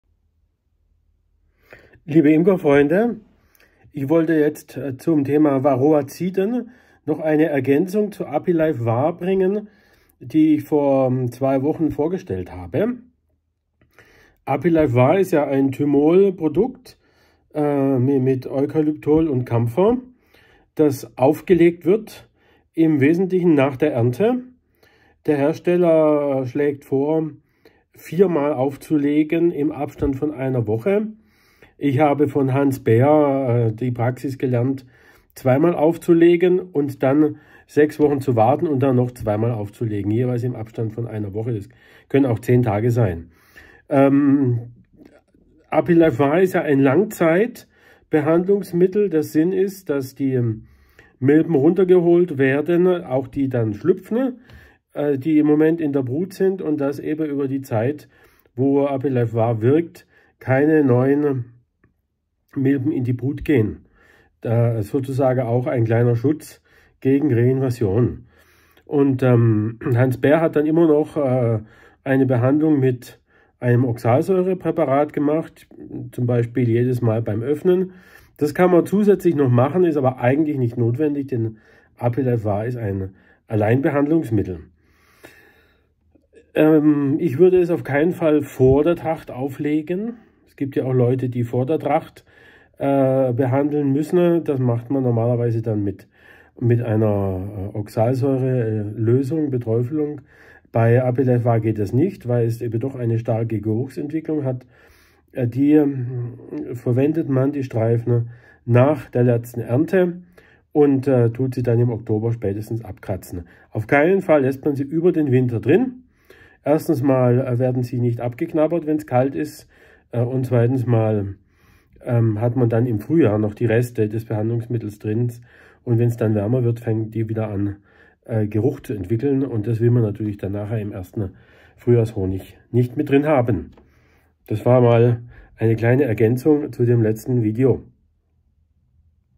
Binder speakt: ApiLife Var wie es Hans Beer verwendet hat.
Jürgen Binder ergänzt das letzte Video um die Verwendung von ApiLife VAR. Wie jedes Varroabehandlungsmittel muß auch ApiLife VAR richtig verwendet werden, damit es keine Rückstände hinterlässt und gut funktioniert. Das Mittel wird nach der letzten Ernte verwendet und drei bis vier mal im Abstand von einer Woche eingesetzt. Hans Beer liess zwischen der zweiten und dritten Behandlung einige Wochen Pause, damit das Volk wieder ungestört ein neues Brutnest aufbauen konnte. Ich verwende neben der totalen Brutentnahme und dem Käfigen der Königin seit vielen Jahren dieses Mittel und bin mit seiner Wirkung äußerst zufrieden. Die Plättchen dürfen aber nicht den Winter über in den Völkern belassen werden. Wenn sie nicht einmal angeknabbert werden wurden sie zu spät aufgelegt.